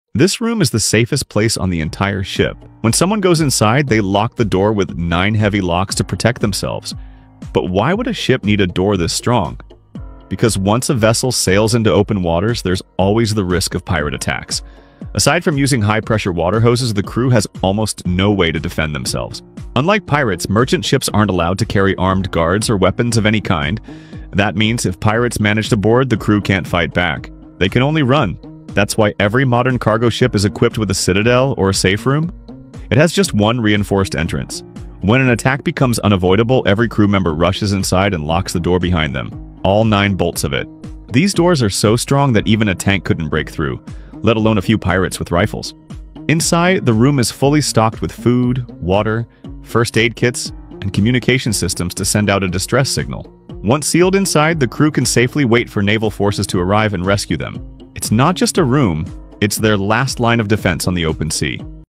The Most Secure Place on a Ship — 9 Locks, Zero Escape! ⚓🔐
On every large cargo ship, there’s a hidden room designed to protect the crew from the unthinkable — pirate attacks, intruders, or emergencies at sea.

This heavily fortified citadel room is sealed behind nine reinforced locks, stocked with food, communication gear, air supply, and emergency controls.

Once inside, the crew can wait safely while authorities work to secure the ship.

It’s the last line of defense on the open ocean.

ship citadel room, maritime safety, anti piracy systems, emergency ship room, secure ship room, cargo ship security, shipping safety